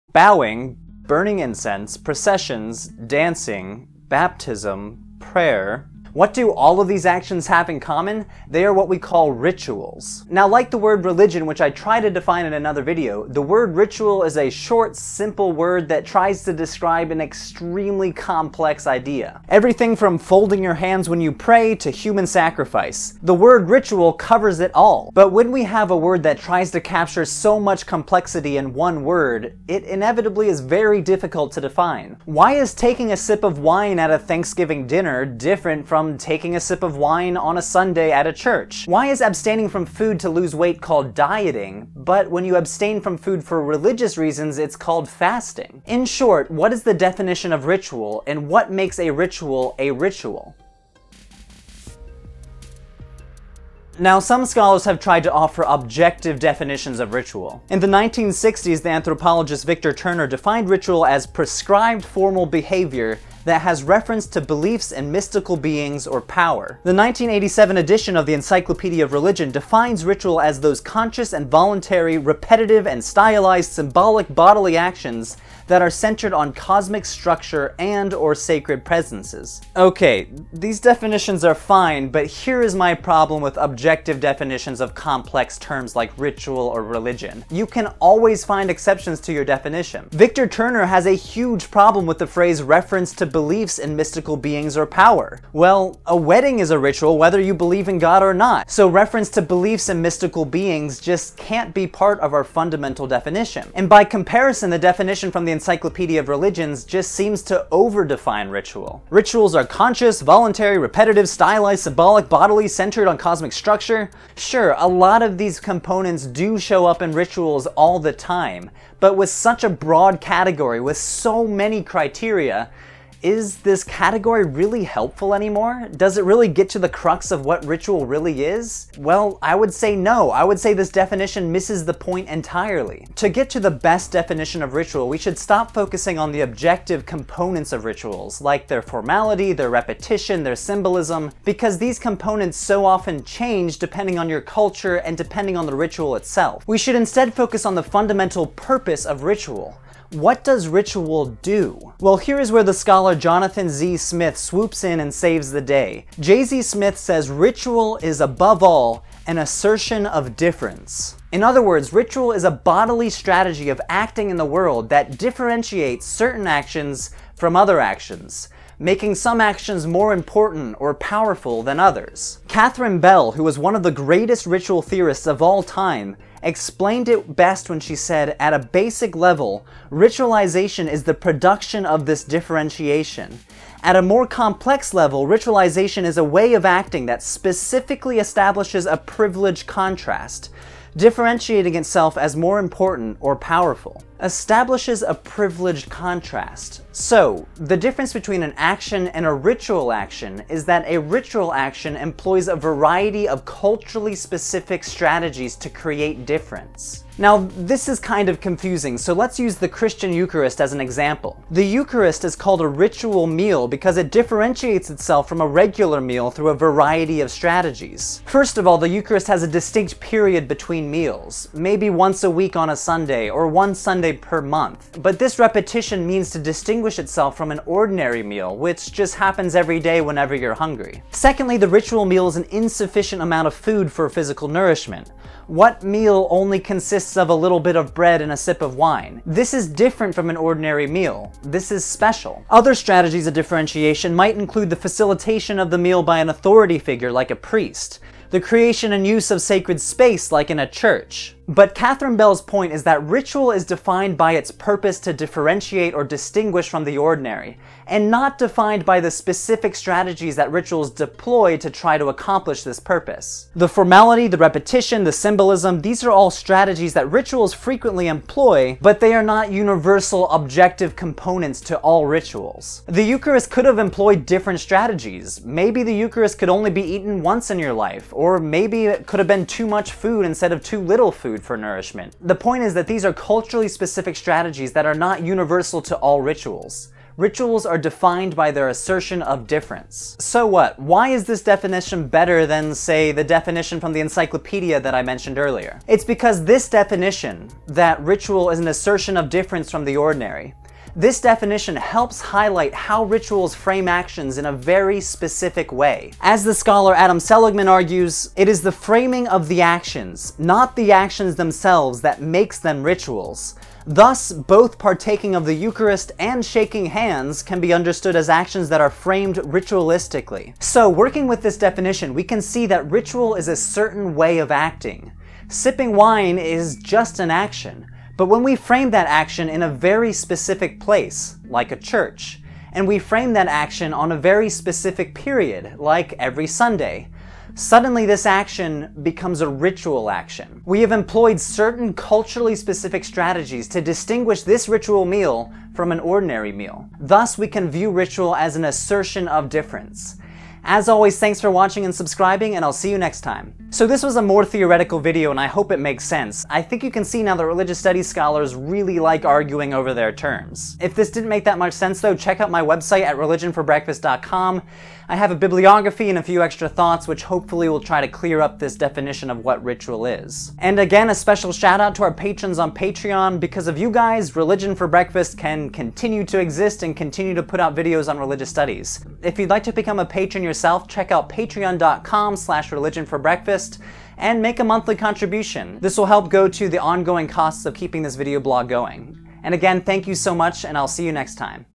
What is Ritual?
"Ritual." We all think we know what it means...right? Ritual, though, is a lot more complicated than you think.

Brian B
Dan D
Alex B
John M
Wilson
Cain D
Manny
Mark H